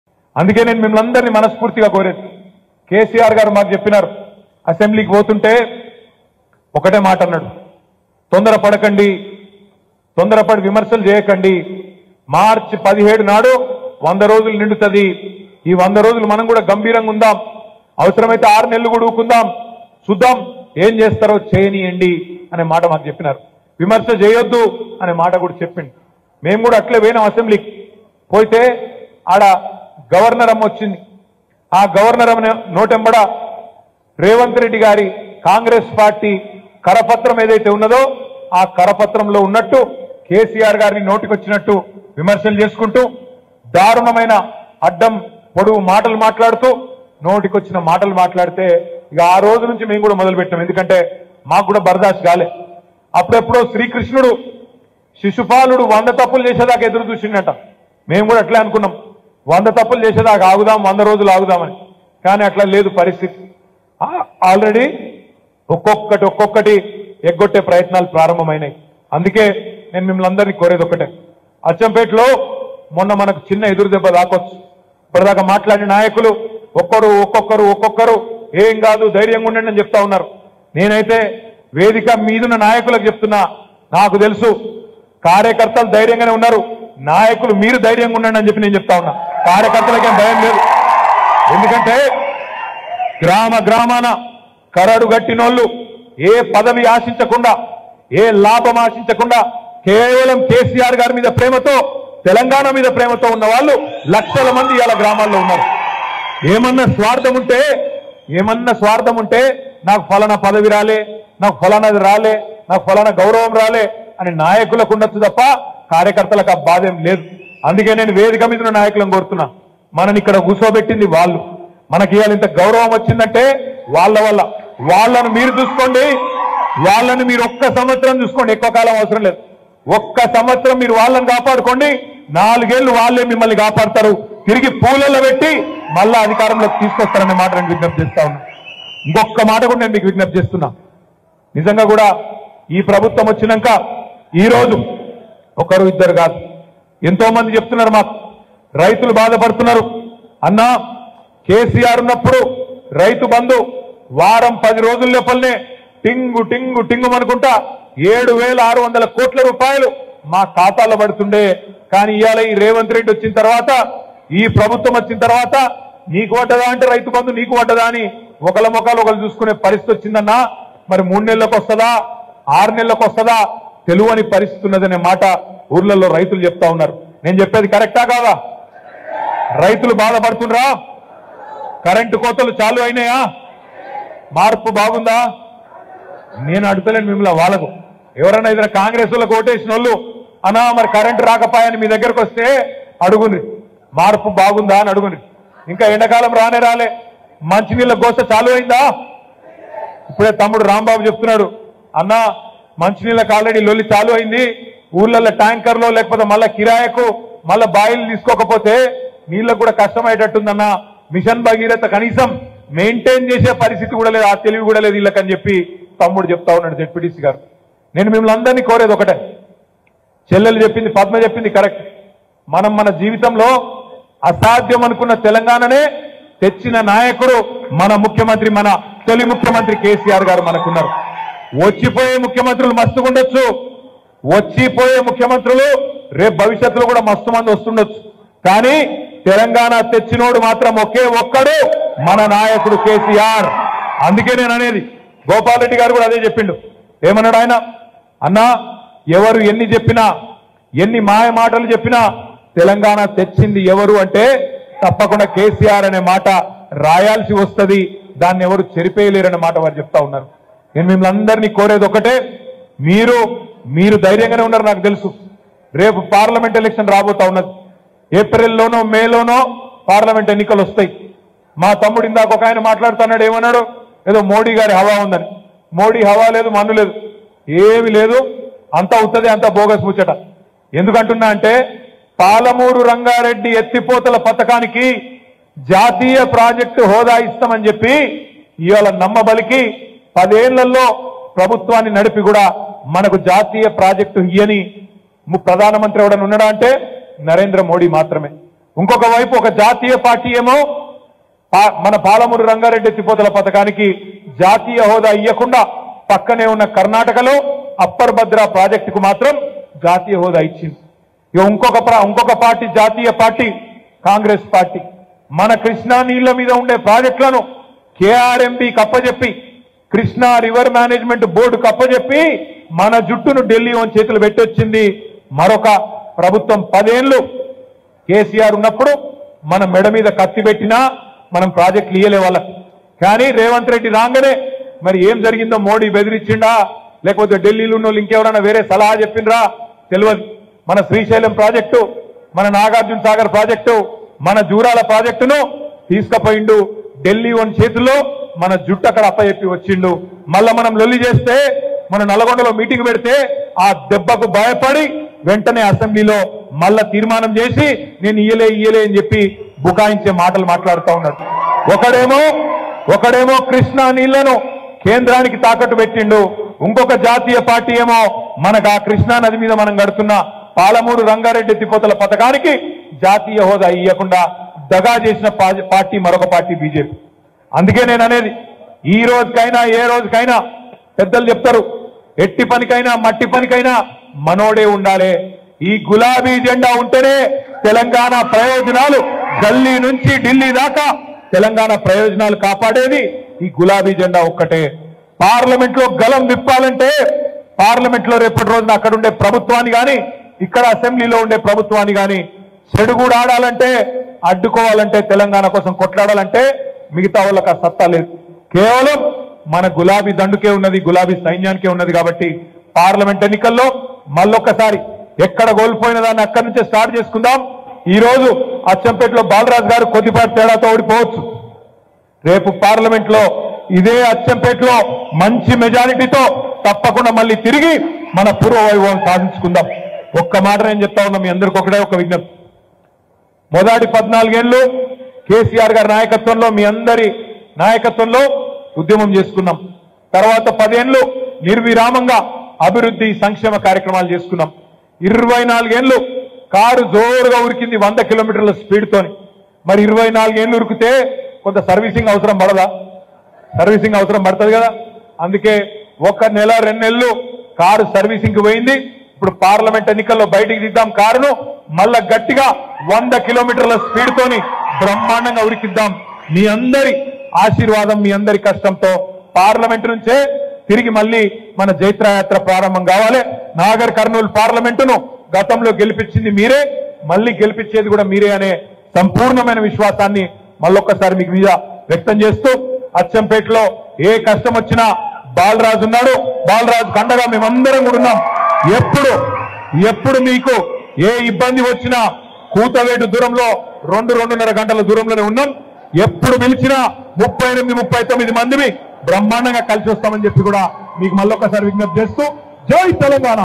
KTR Powerfull Comments on CM Revanth At Achampet Meeting | BRS | Congress | Mirror TV Plus
ఒకరు ఏమో కృష్ణా జలాలు కేంద్రానికి తాకట్టు పెట్టాడు
ఇంకొకరు ఏమో...? | KTR Powerfull Comments on CM Revanth At Achampet Meeting | BRS | Congress | Mirror TV Plus